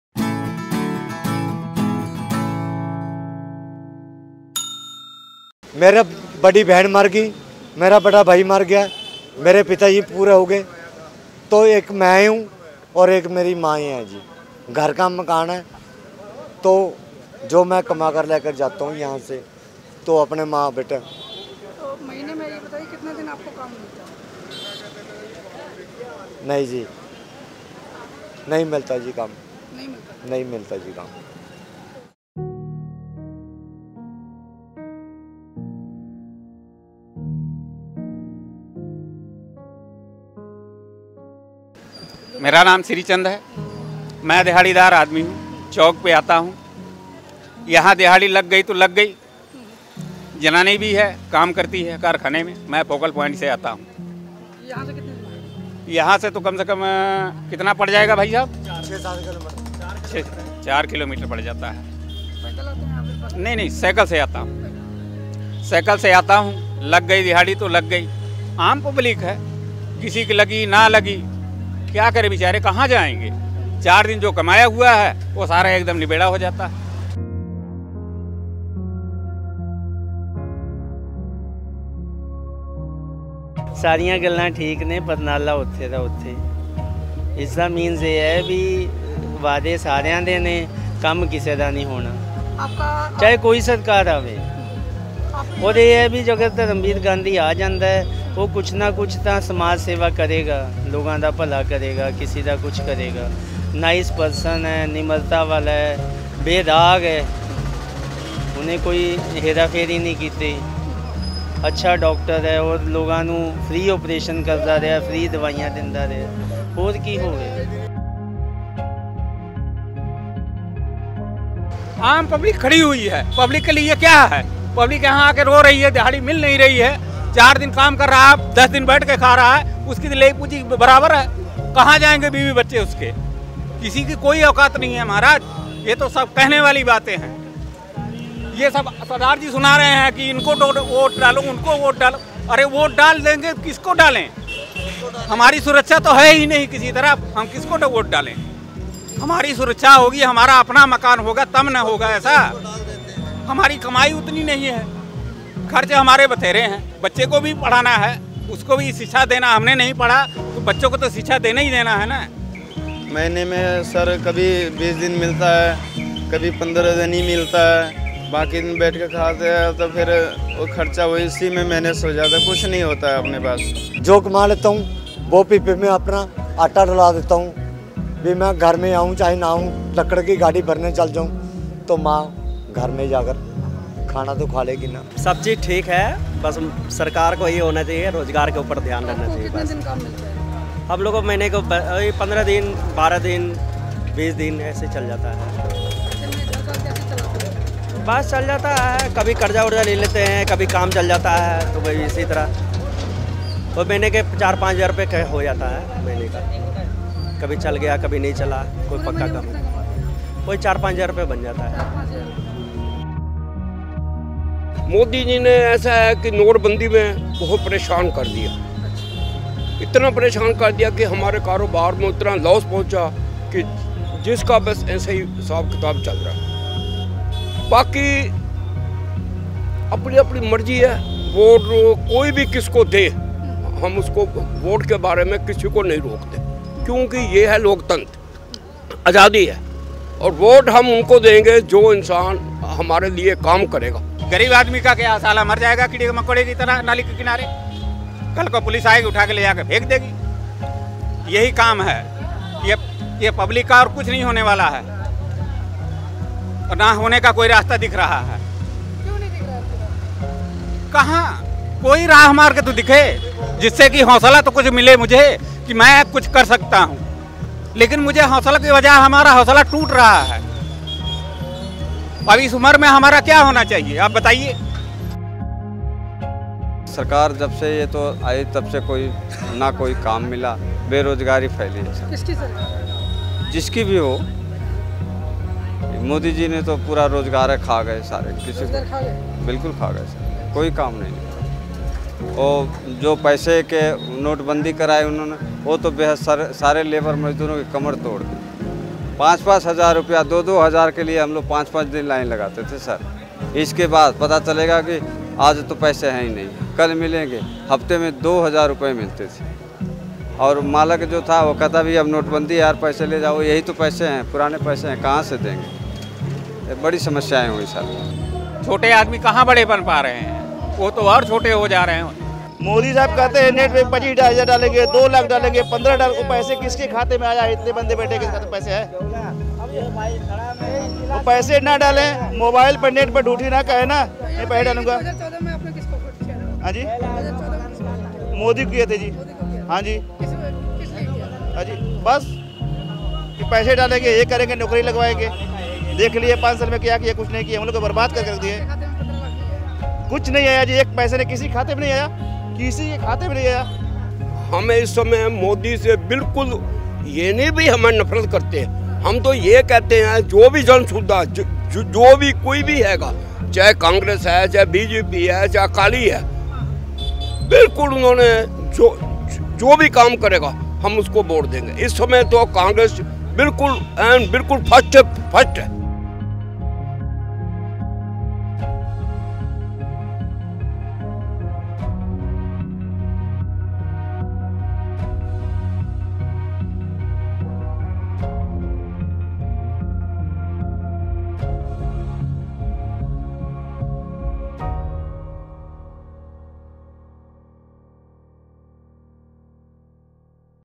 Wait, Despair and Hope: Fate of Daily Wage Labourers in Punjab
Punjab, once one of the prosperous states in India, is now reeling under a crisis. Lack of jobs and demonetisation  have crippled the lives of daily wagers, whose families now depend on micro loans to run  households. They  squarely blame the central government for their plight. NewsClick talked to daily wage workers in Patiala to find out what they feel about politics and elections.